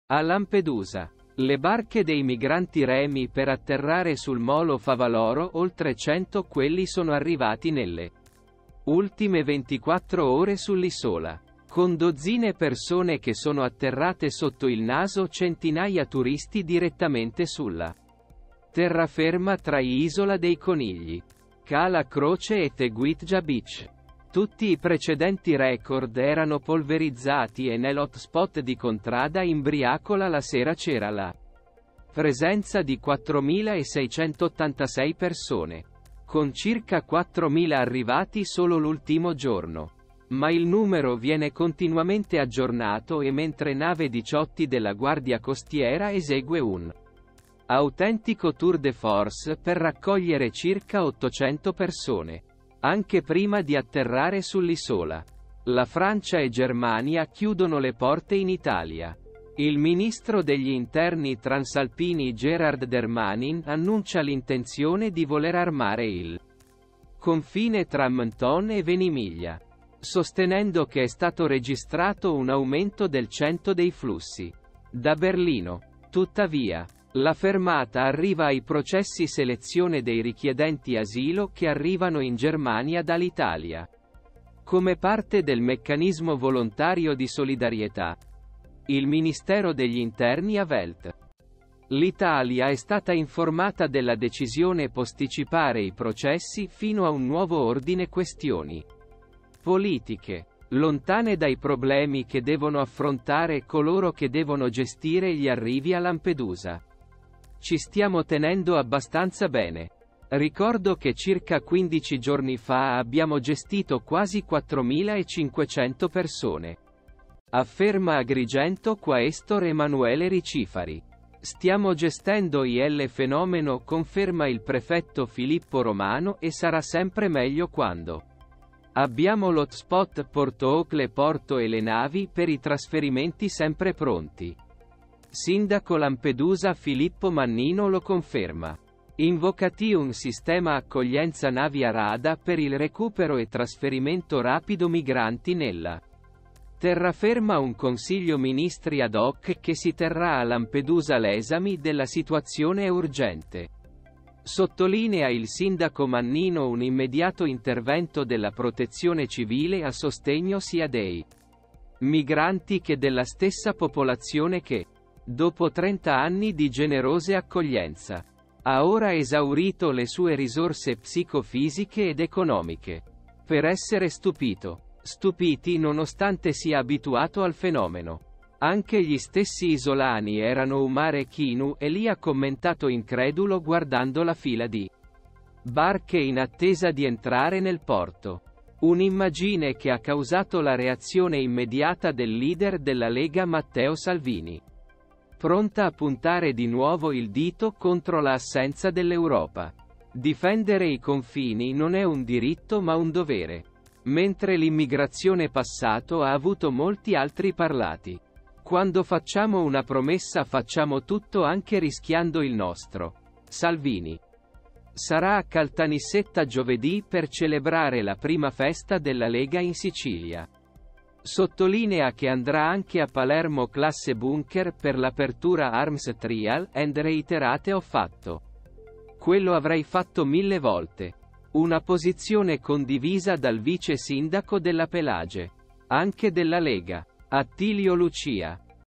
Lampedusa, record di atterraggi.
A Lampedusa, le barche dei migranti remi per atterrare sul molo Favaloro oltre cento quelli sono arrivati nelle ultime 24 ore sull'isola, con dozzine persone che sono atterrate sotto il naso centinaia turisti direttamente sulla terraferma tra iIsola dei conigli, Cala Croce e The Guitgia Beach. Tutti i precedenti record erano polverizzati e nell'hotspot di Contrada Imbriacola la sera c'era la presenza di 4.686 persone, con circa 4 mila arrivati solo l'ultimo giorno. Ma il numero viene continuamente aggiornato e mentre nave Diciotti della Guardia Costiera esegue un autentico tour de force per raccogliere circa 800 persone, anche prima di atterrare sull'isola, la Francia e Germania chiudono le porte in Italia. Il ministro degli interni transalpini GérarldDermanin annuncia l'intenzione di voler "armare" il confine tra Menton e Venimiglia, sostenendo che "è stato registrato un aumento del 100% dei flussi. Da Berlino, tuttavia, la fermata arriva ai processi selezione dei richiedenti asilo che arrivano in Germania dall'Italia come parte del "meccanismo volontario di solidarietà".Il ministero degli interni a "Welt", l'Italia è stata informata della decisione "posticipare" i processi "fino a un nuovo ordine"  Questioni politiche, lontane dai problemi che devono affrontare coloro che devono gestire gli arrivi a Lampedusa. "Ci stiamo tenendo abbastanza bene, ricordo che circa 15 giorni fa abbiamo gestito quasi 4.500 persone", afferma Agrigento Quaestor Emanuele Ricifari. "Stiamo gestendoIl fenomeno - conferma il prefetto Filippo Romano - e sarà sempre meglio quando abbiamo l'hotspot Portoocle Porto e le navi per i trasferimenti sempre pronti. sindaco Lampedusa Filippo Mannino lo conferma, invocati"Un sistema accoglienza navi a RADA per il recupero e trasferimento rapido migranti nella terraferma un consiglio ministri ad hoc che si terrà a Lampedusa l'esame della situazione " "È urgente sottolinea il sindaco Mannino - un immediato intervento della protezione civile a sostegno sia dei migranti che della stessa popolazione che, dopo 30 anni di generose accoglienza, ha ora esaurito le sue risorse psico -fisiche ed economiche". Per essere stupito, stupitiNonostante sia "abituato" al fenomeno, anche gli stessi isolani erano "U Mare Chinu è lì " ha commentato incredulo guardando la fila di barche in attesa di entrare nel porto. Un'immagine che ha causato la reazione immediata del leader della lega Matteo Salvini, pronta a puntare di nuovo il dito contro la "assenza dell'Europa". Difendere i confini non è un diritto ma un dovere. Mentre l'immigrazione passato ha avuto molti altriParlati, quando facciamo una promessa facciamo tutto anche rischiando il nostro ".Salvini, sarà a Caltanissetta giovedì per celebrare la prima festa della lega in Sicilia, sottolinea che andrà anche a Palermo classe Bunker per l'aperturaArms Trial and Reiterate "Ho fatto quello avrei fatto mille volte". Una posizione condivisa dal vice sindaco della Pelagie, anche della lega, Attilio Lucia, lancia un appello "Devi trovare misure di accoglienza alternative.Mi rivolgo alle istituzioni nazionali ed europee che non hanno la minima idea di ciò è accaduto oggi a Lampedusa. Ti invito a venire a vedere con i tuoi occhi. Non possiamo più e non vogliamo più prenderci in carico questo fenomeno migratorio, mentre l'Unione europea rimane miope di fronte a un evento che rimarrà nella storia ". Anche l'arcivescovo Agrigento, Alessandro Damiano, afferma essere preoccupato situazione Lampedusa "Mi rivolgo a un sentito appello per intervenire con preoccupazione tempestiva, garantire gestione più regolare dei flussi migranti in transito verso l'isola una direzione direzioneQuesto è altrettanto rispettoso, così tanto coloro che nella disperazione e nel bisognoso cercano rifugio nelle nostre coste, entrambi coloro che con uno spirito umanitario, un senso civico e la carità cristiana sono sempre stati pronti ad accoglierli, ma ora noA più forza prenderne cura da solo ".